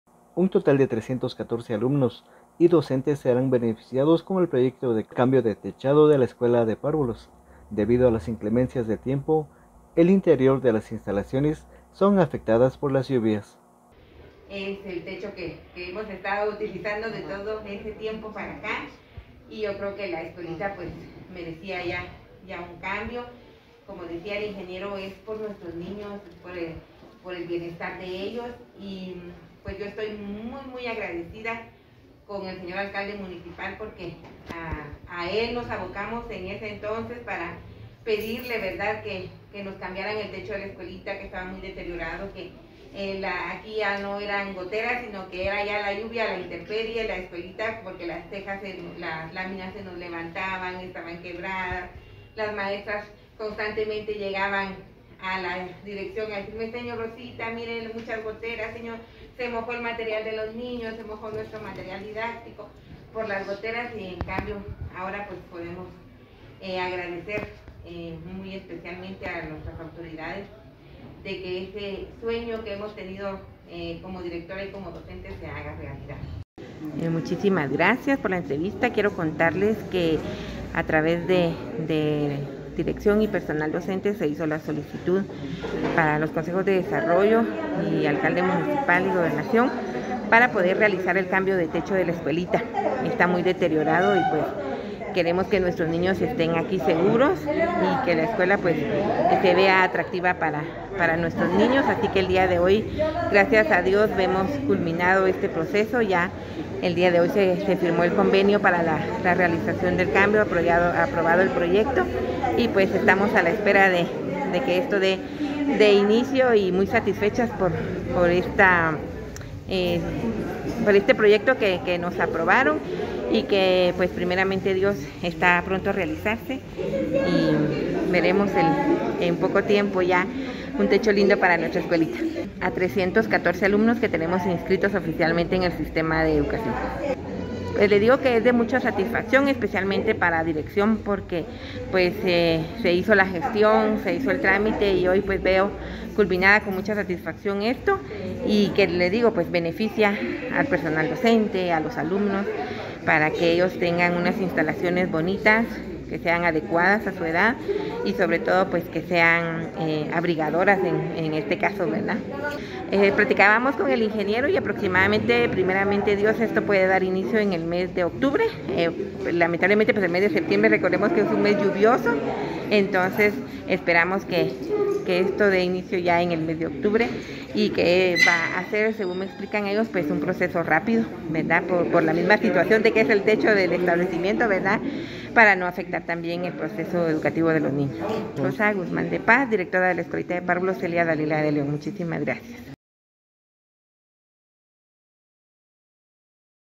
ALUMNOS Y DOCENTES BENEFICIADOS POR CAMBIO DE TECHADO
Un total de 314 alumnos y docentes serán beneficiados con el proyecto de cambio de techado de la escuela de párvulos, debido a las inclemencias del tiempo, el interior de las instalaciones son afectadas por las lluvias.